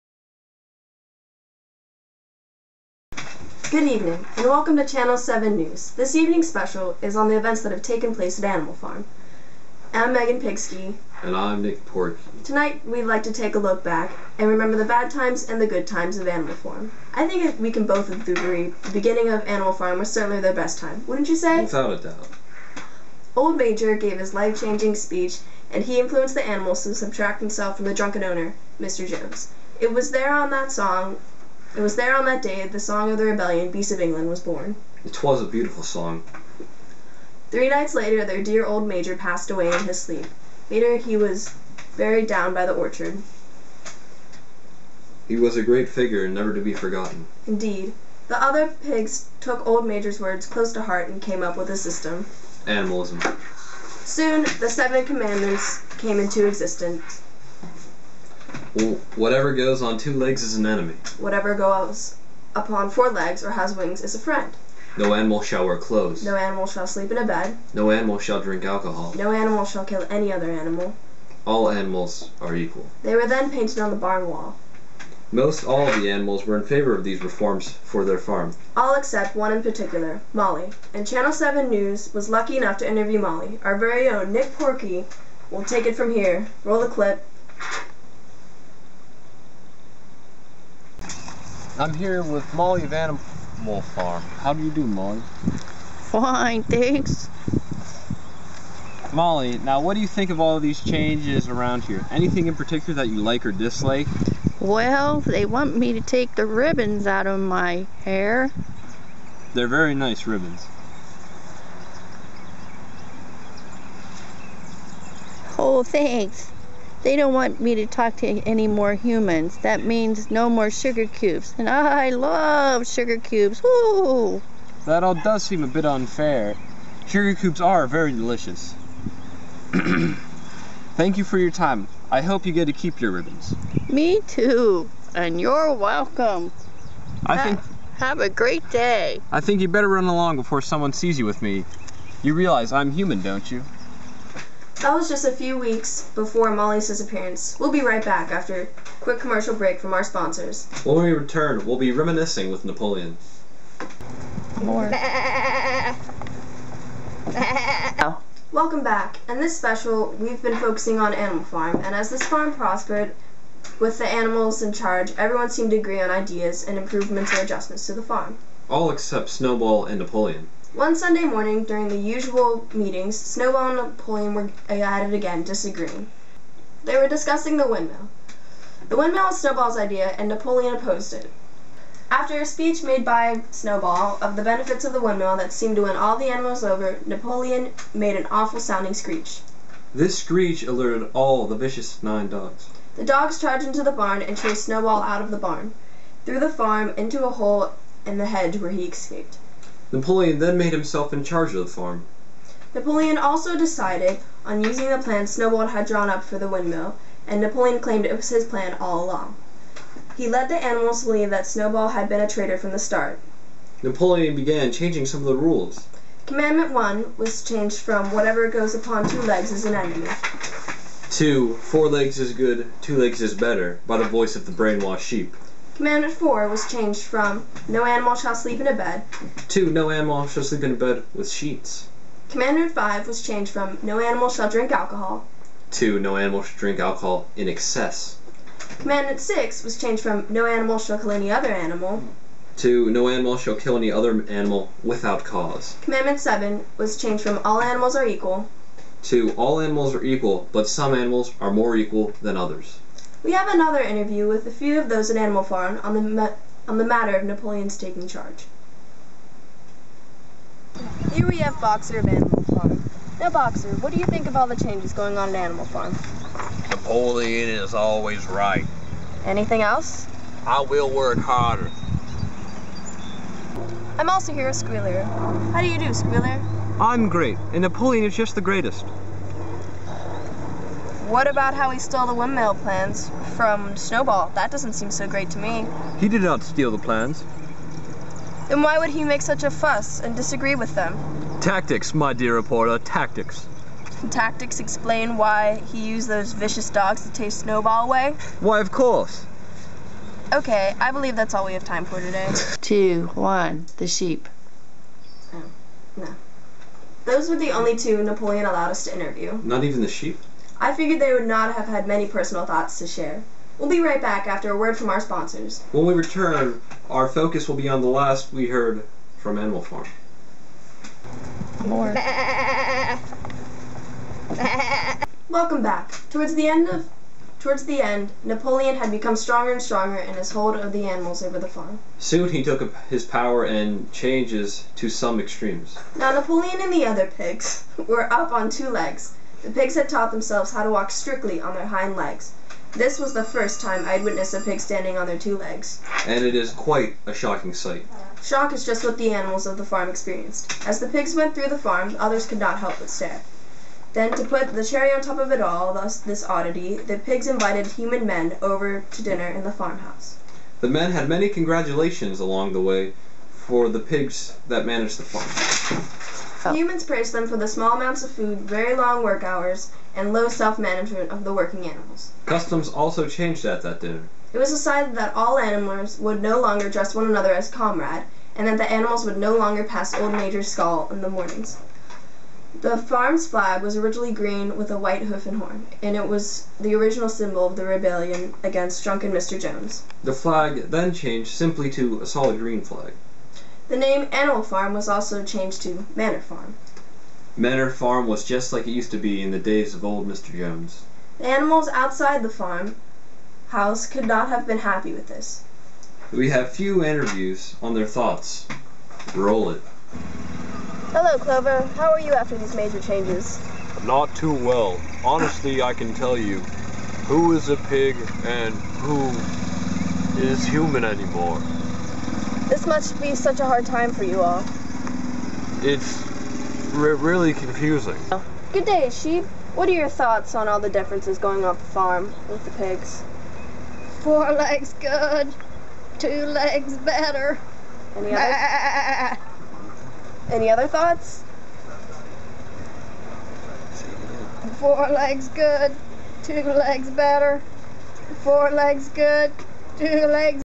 Channel 84 News Team live from Manor Farm
A school project summarizing the major plot points and themes of the George Orwell classic, Animal Farm.  Set as a fictional newscast reporting on the events that took place in the preceding years on Manor Farm (formerly Animal Farm).  A must watch!